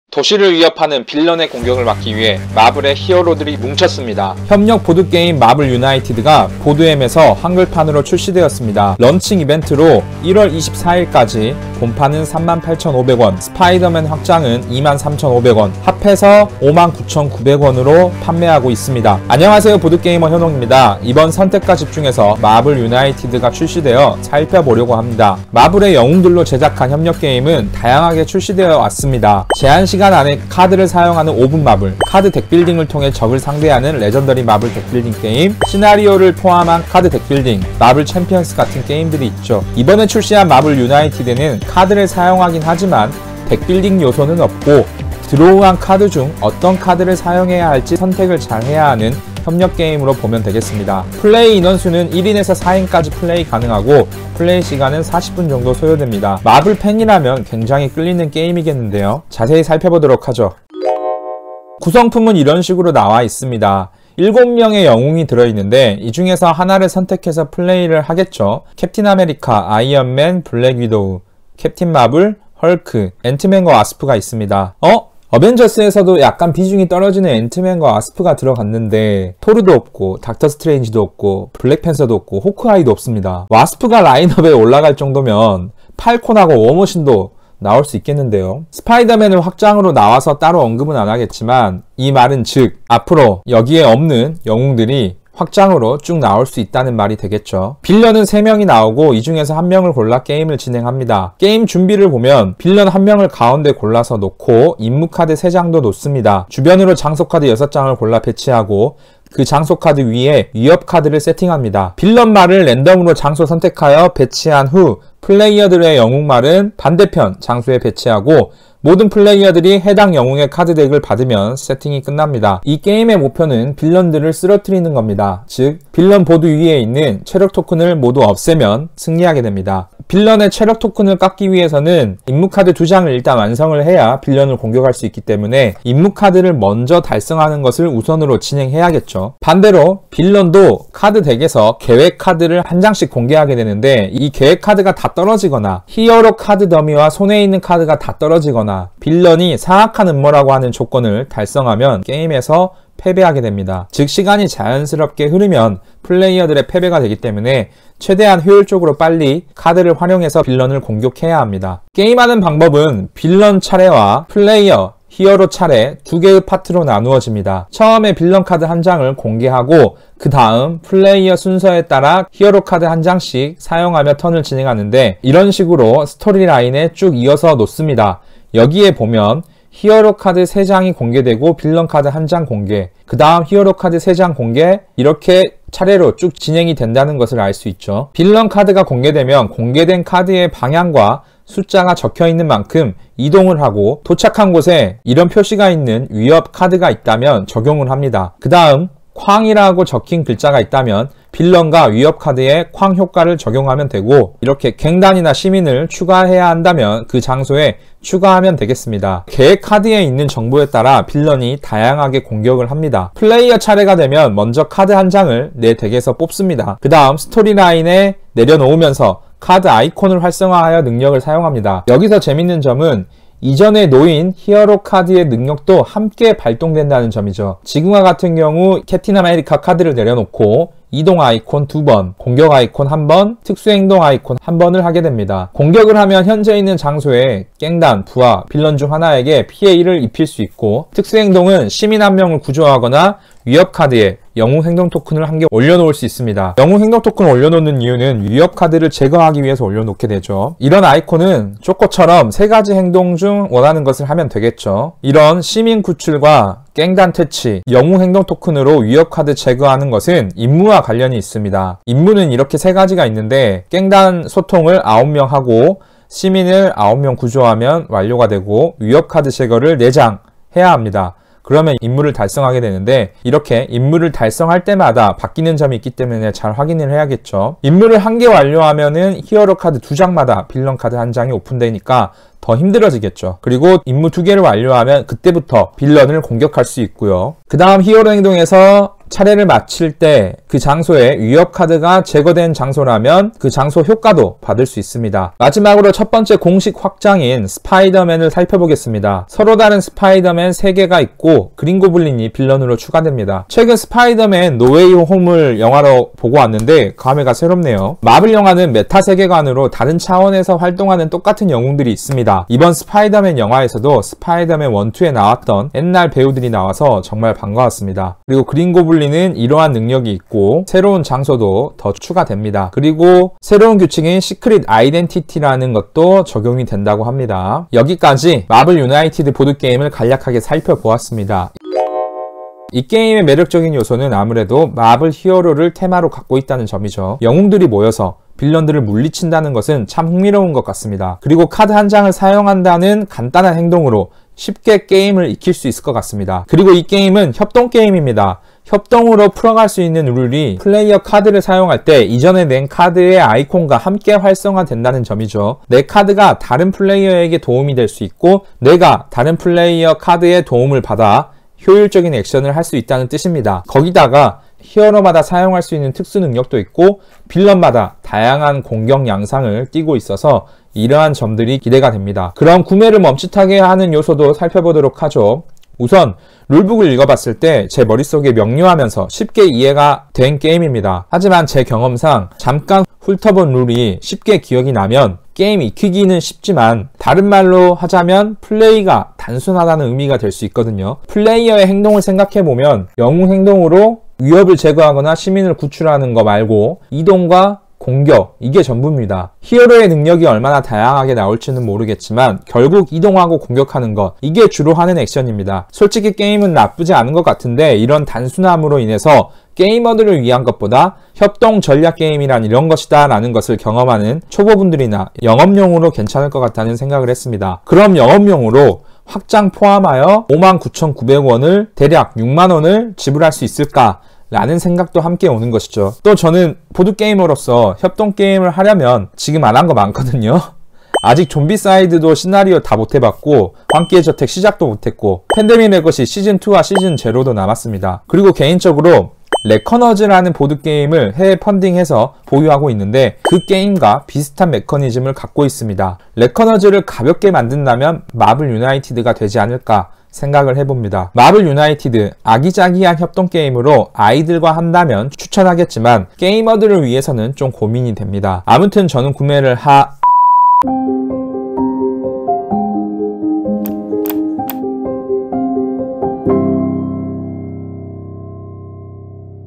[현옹] '마블 유나이티드' 협동 게임 살펴보기
보드게이머 현옹의 새로운 프로젝트 선택과 집중

사고 싶은 보드게임은 많고, 돈은 없고, 보관할 공간도 부족한데 어떻게 해야 하지? 고민하고 있는 요즘 대 한글화 시대로 무지성 구매를 멈추고, 보드게임을 따져보고 사기로 했습니다. 그래서 플레이를 한번도 해보지 않았지만 정보만 살펴보고 함께 살지말지 고민하는 시간

이번 영상에서는 '마블 유나이티드'를 살펴보겠습니다.

00:00 마블유나이티드 출시 소식
01:01 어떤 게임인가
05:31 게임에 대한 생각